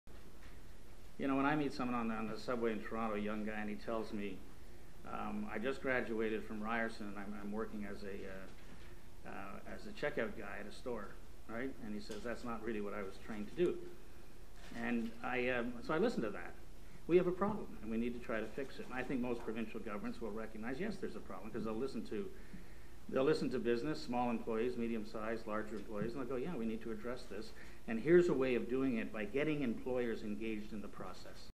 Jim Flaherty on skills retraining
Finance Minister Jim Flaherty talks to reporters about the Canada Job Grant, which changes how Ottawa doles out training funds to help unemployed workers learn new skills.